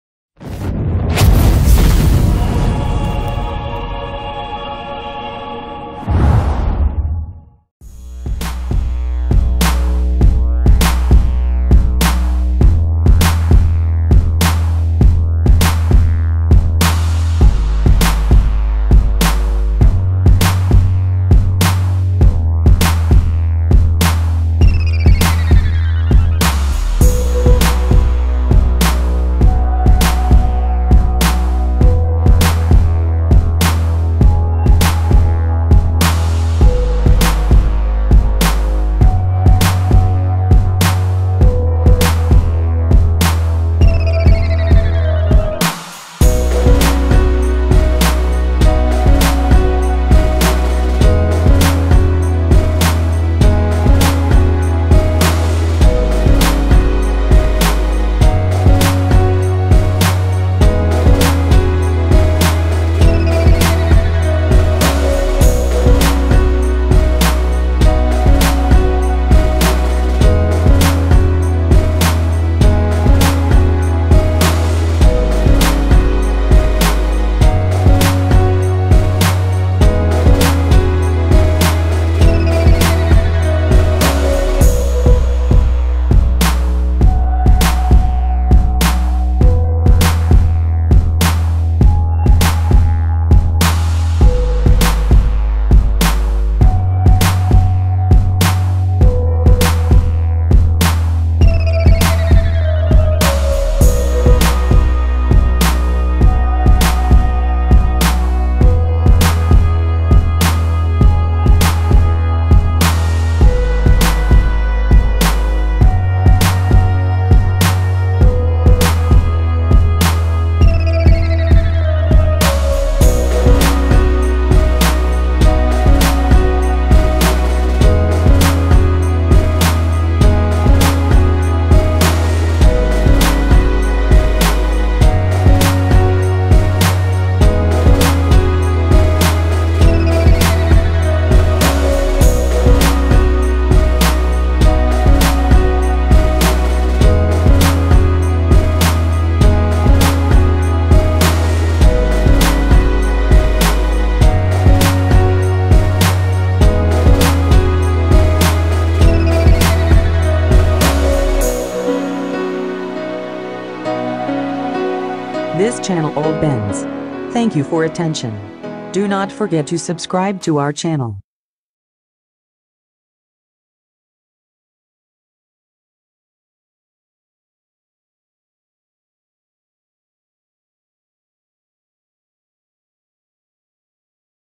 2019 Mercedes-AMG GT Four-Door spy shots
Mercedes-AMG GT Four-Door is a serial version of the Mercedes-AMG GT Concept, which was introduced in March 2017 in Geneva. The car is based on the MRA platform and will be presented in early 2018. Under the hood will accommodate a hybrid power plant, which includes a gasoline turbo engine and an electric motor. The total power of the power plant will be more than 700 hp, and the transmission will be all-wheel drive with the ability to control the ratio of the distribution of the moment along the axes.